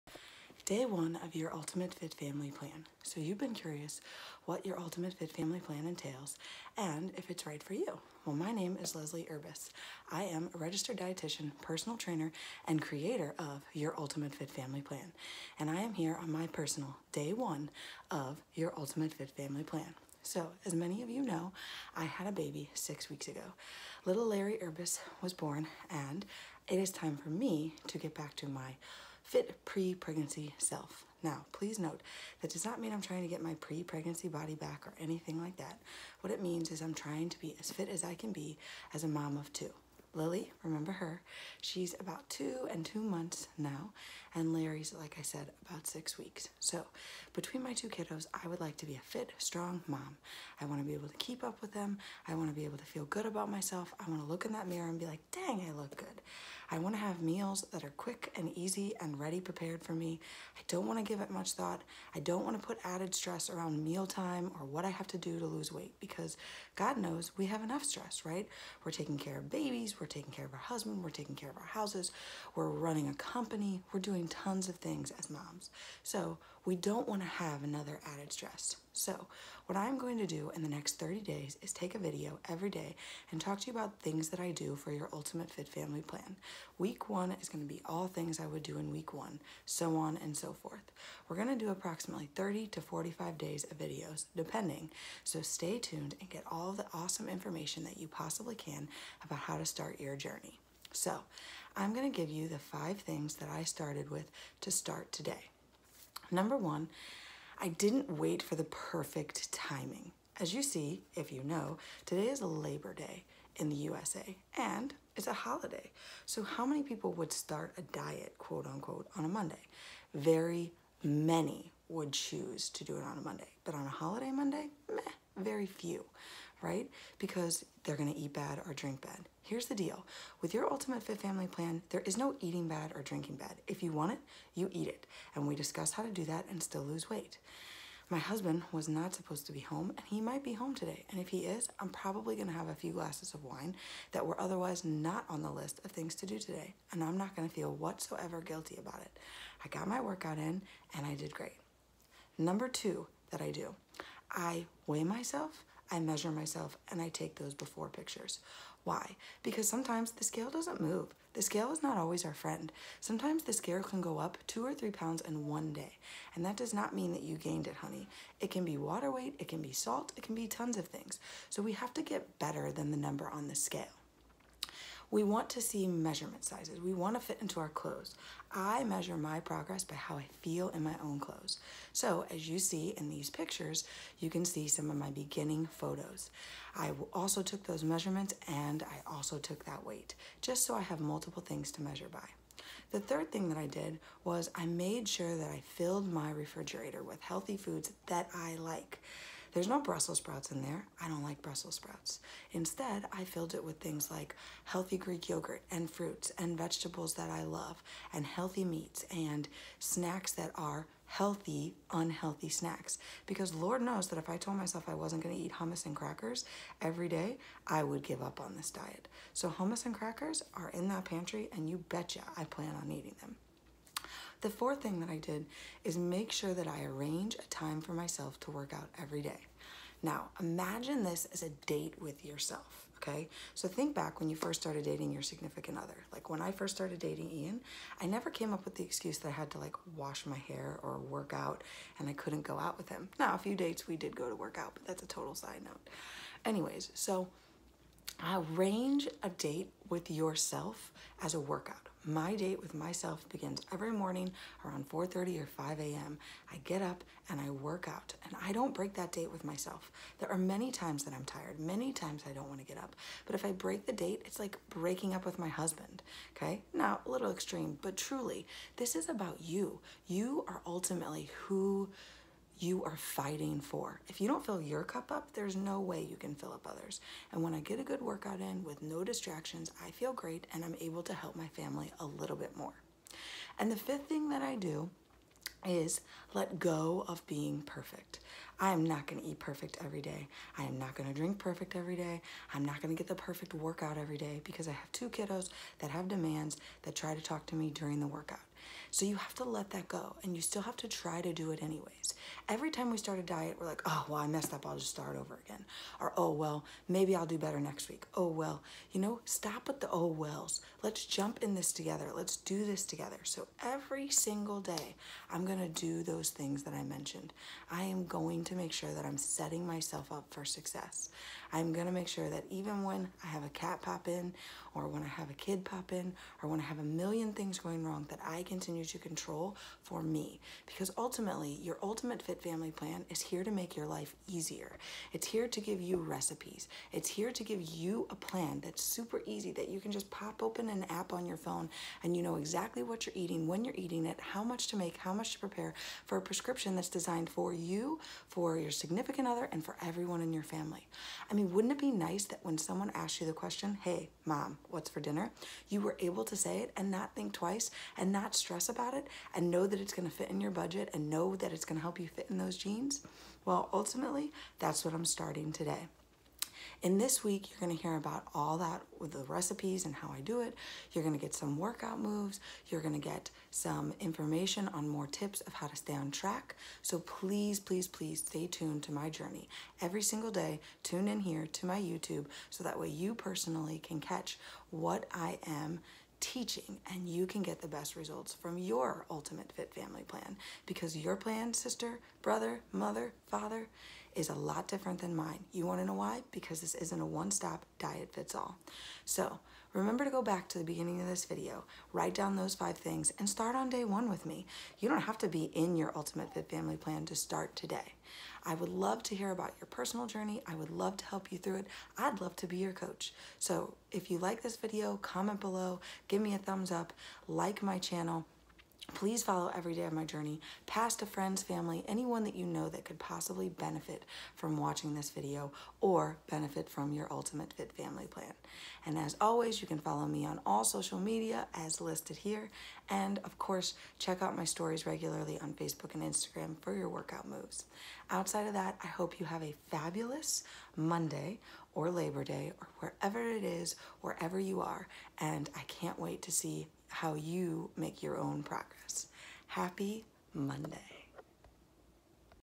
PostPartum Weight Loss Journey.
Watch my personal weight loss journey after baby number 2.

In this video you will see my top 5 tips to starting your program with Your Ultimate Fit Family plan.

Watch the next 30-45 days of videos to see one momma begin her new postpartum journey.

Your weight loss, fitness, and nutrition expert for the whole family
Here to help your family fuel for life and not for FAD.